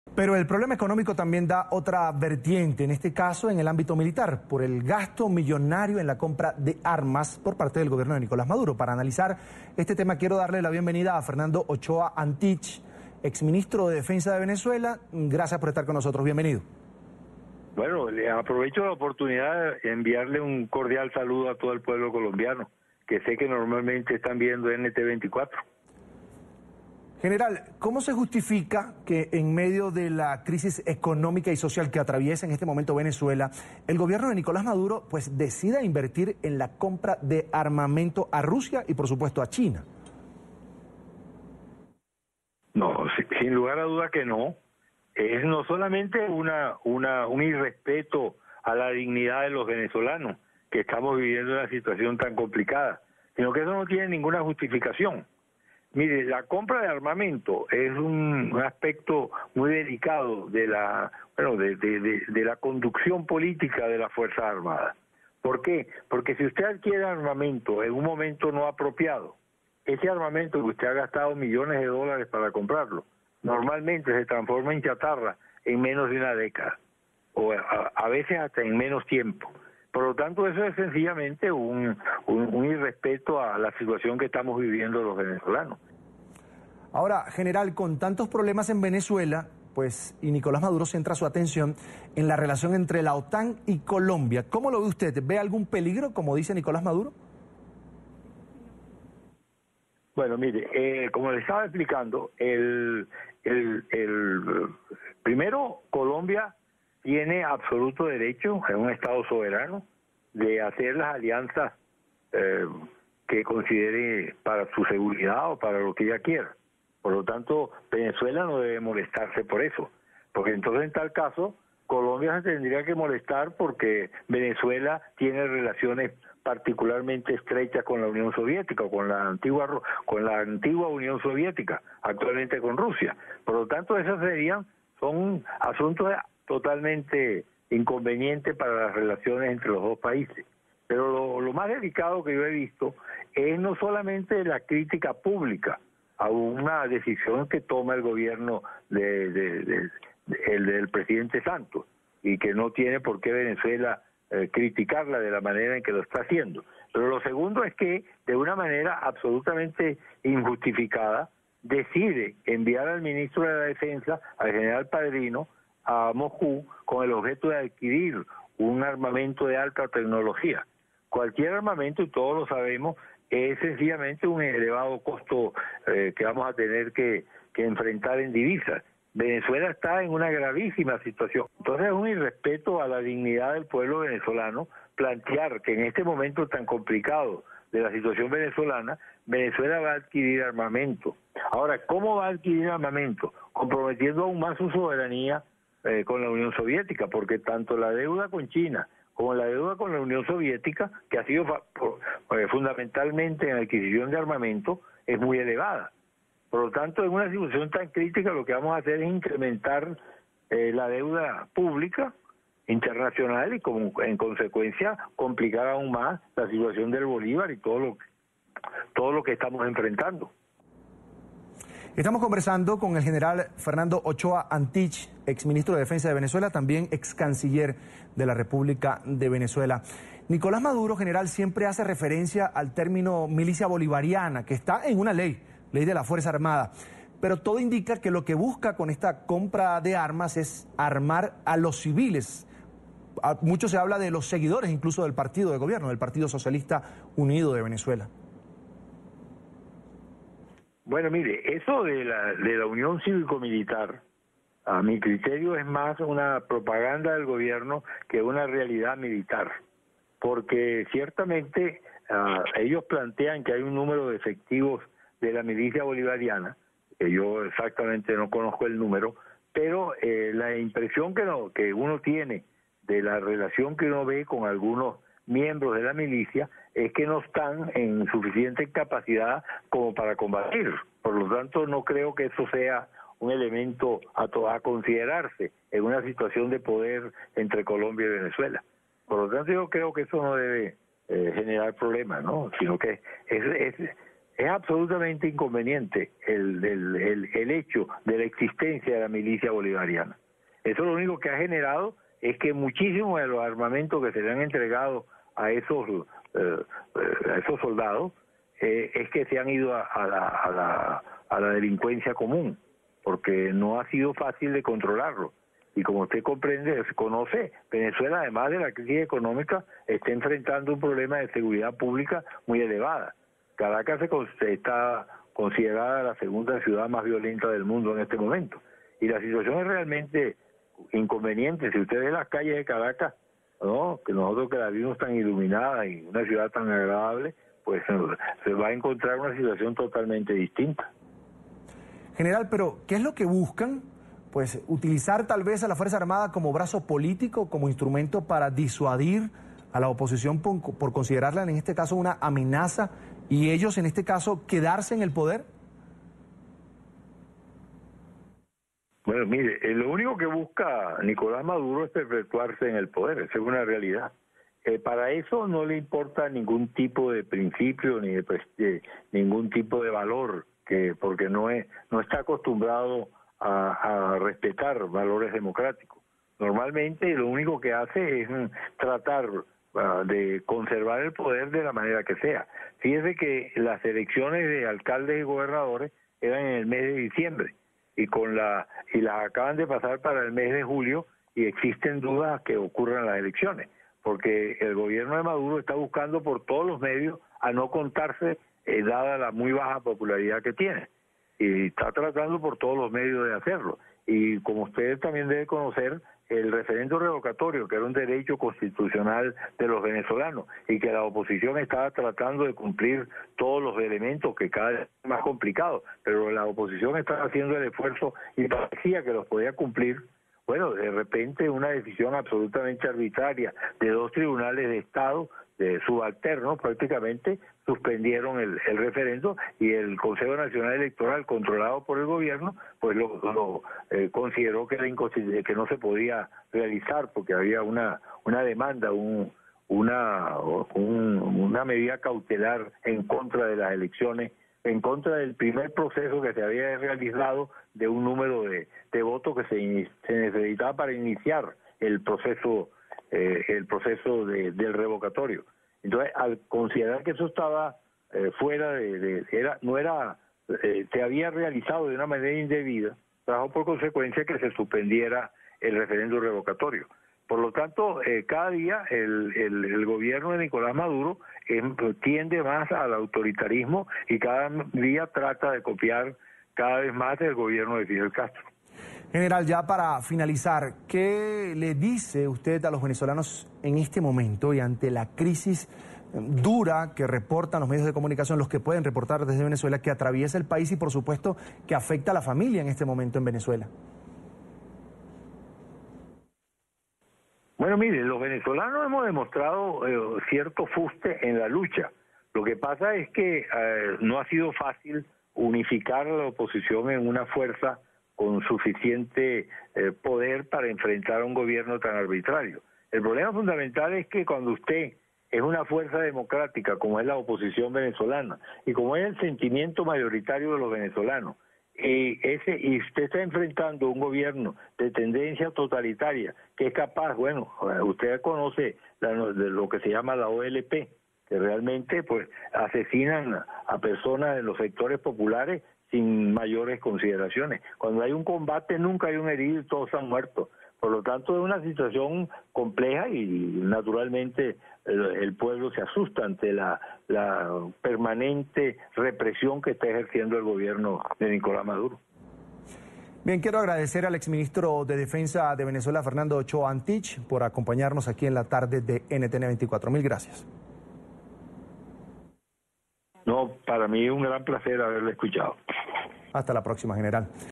"Compra de armas es un irrespeto a la dignidad venezolana": Fernando Ochoa Antich a NTN24
El militar retirado Fernando Ochoa Antich, calificó como un irrespeto a la dignidad de los venezolanos la compra de armas anunciada por el gobierno de Nicolás Maduro a Rusia y China.